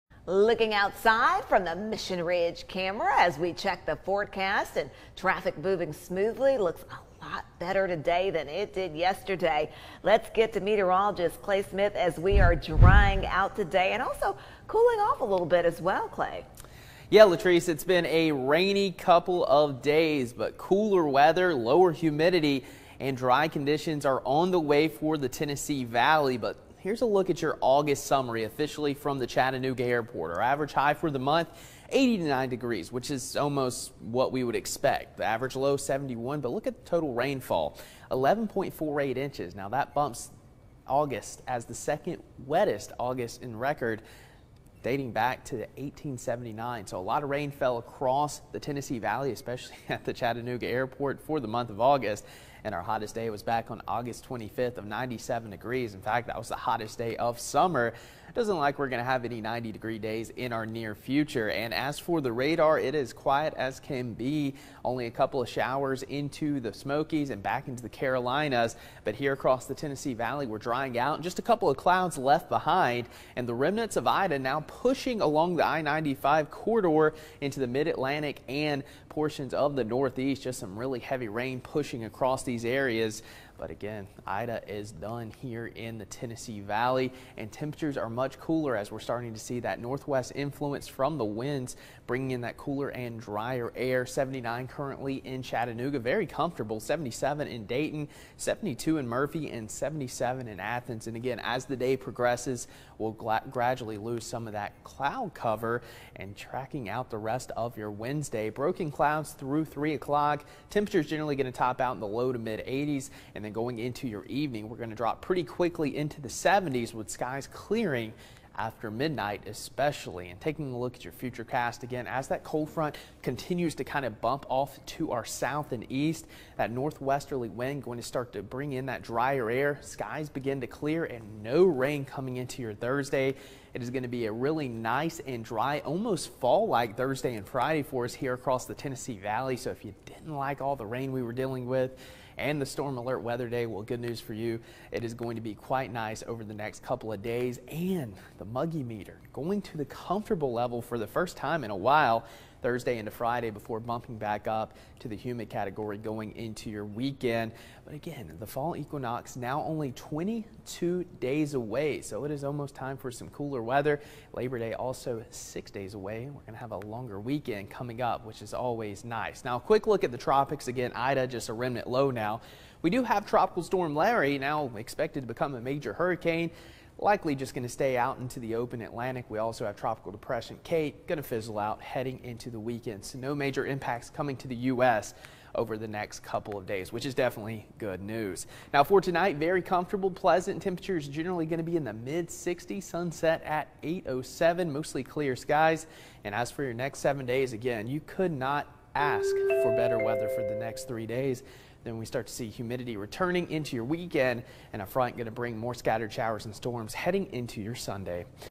Clay Smith's midday weather
A cold front will sweep through this afternoon into evening fostering an even greater drop in humidity.